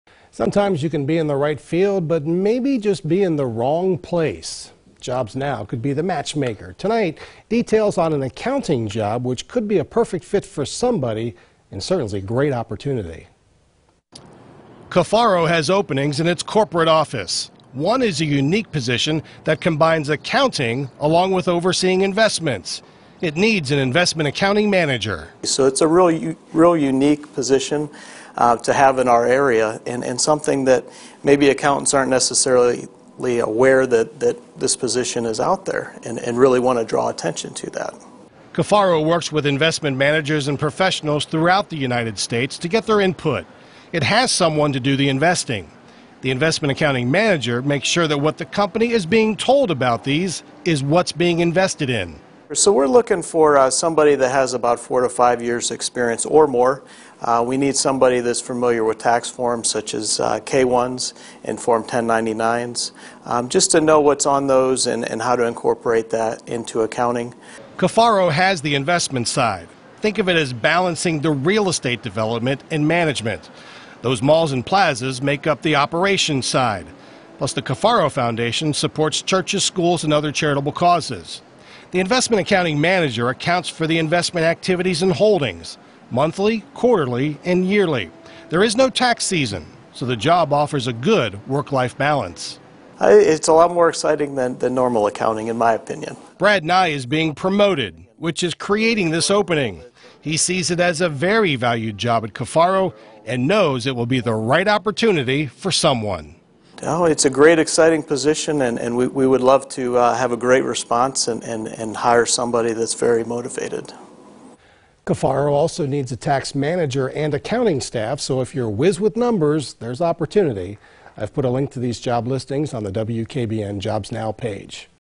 JobsNOW: Cafaro Company in need of investment accounting manager
The Cafaro Company has openings in its corporate office. One is a unique position that combines accounting with overseeing investments. The company needs an investment accounting manager.